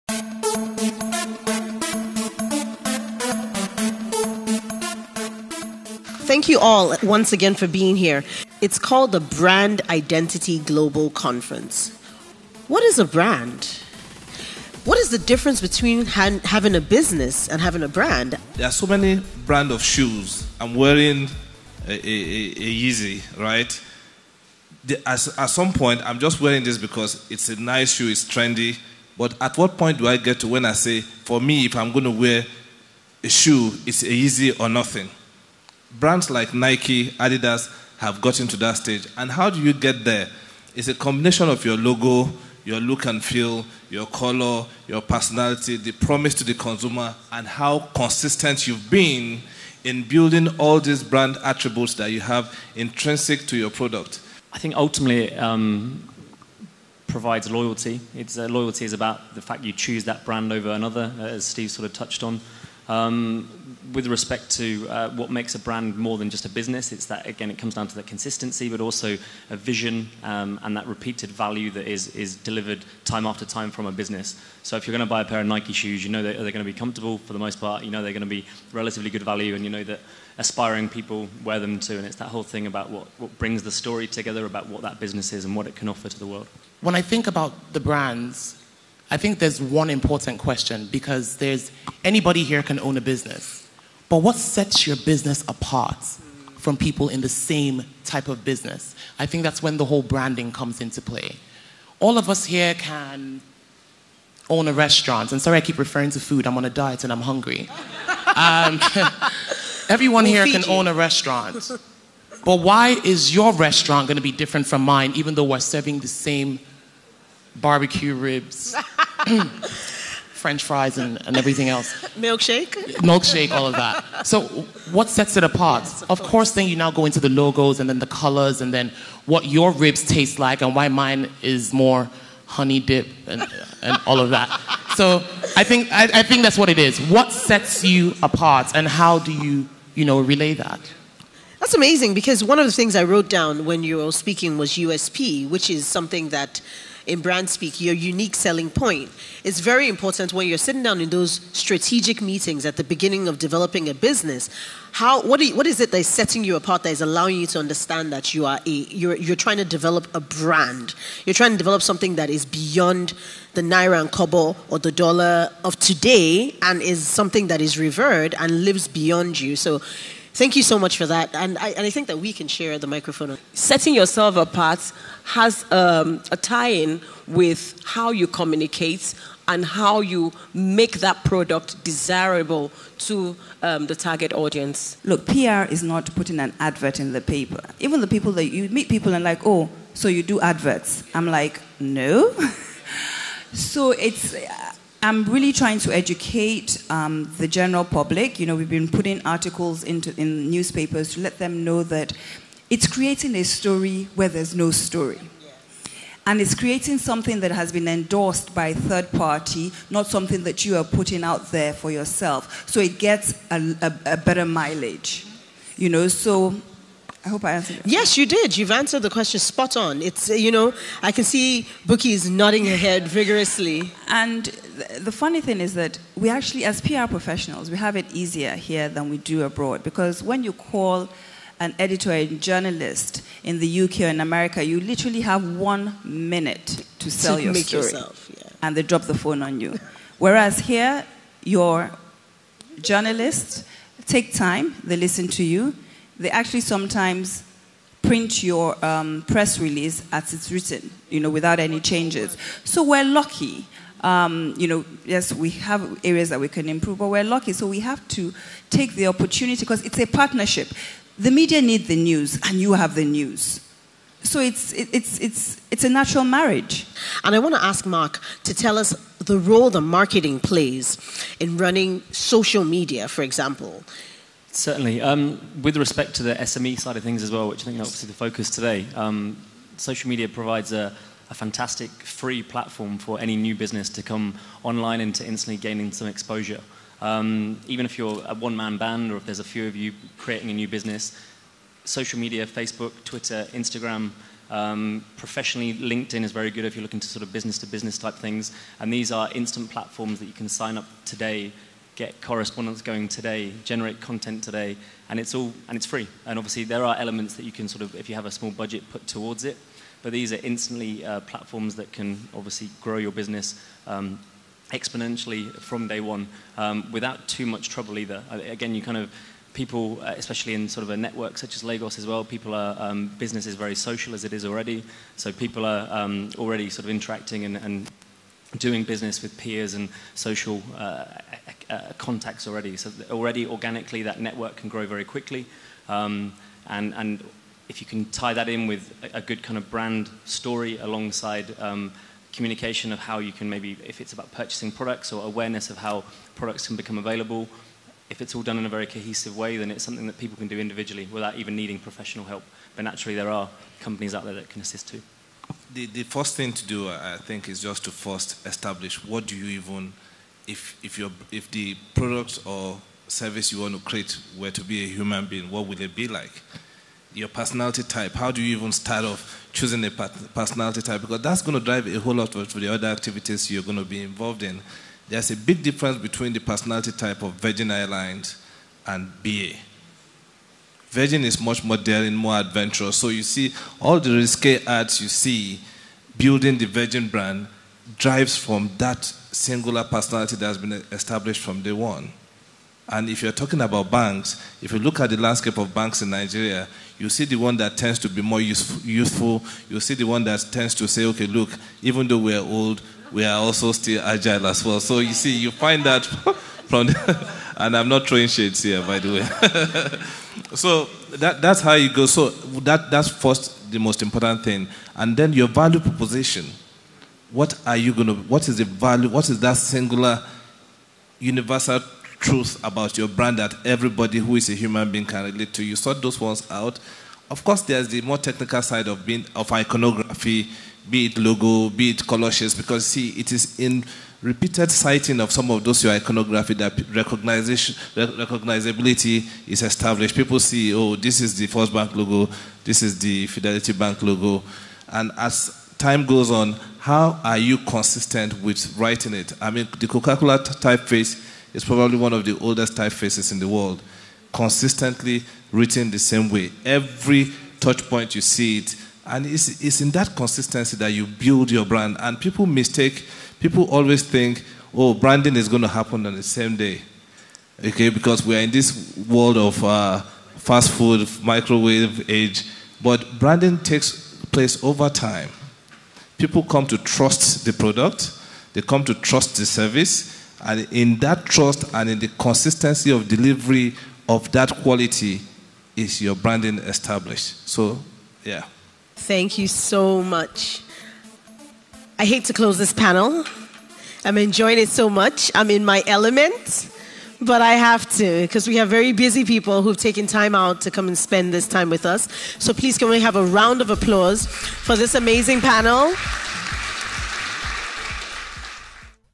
The Showcase - Building Your Brand
The Brand Identity Global (BIG) Conference helps Small Businesses to Think BIG and Start Small. Watch this segment on how to build your brand with Steve Babaeko, Bukky Karibi-Whyte, Mark Slade and Tokunbo George-Taylor.